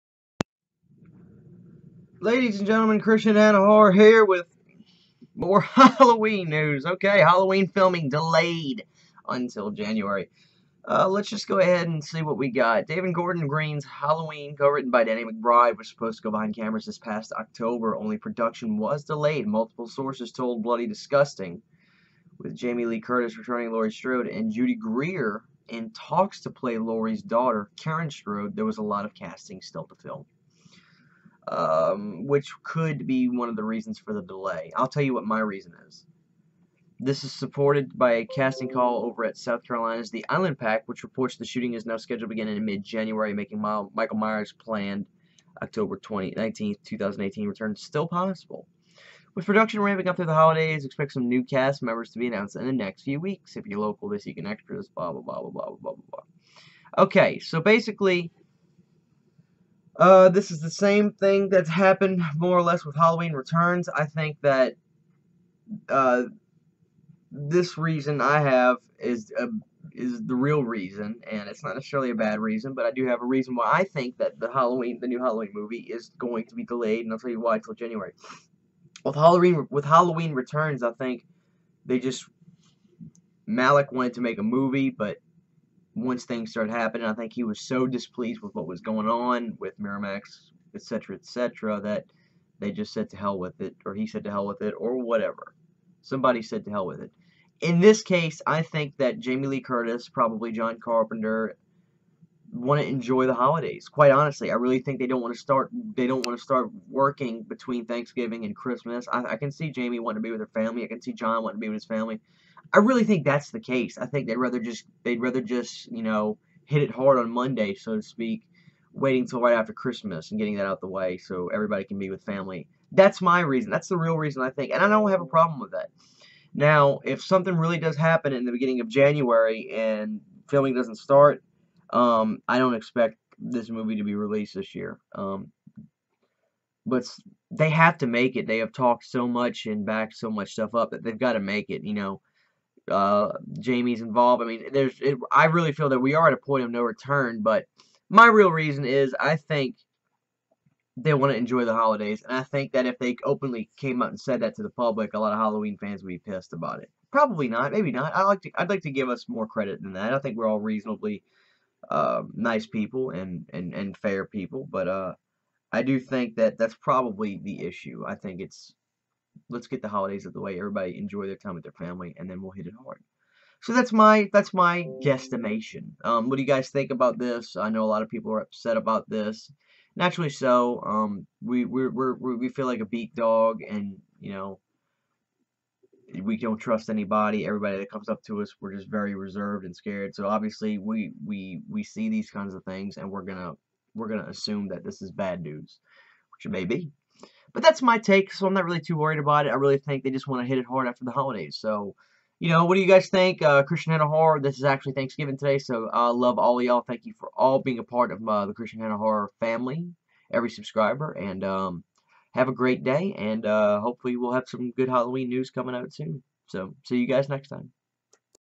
Halloween filming DELAYED Until January
The new halloween film will not start filming until january of 2018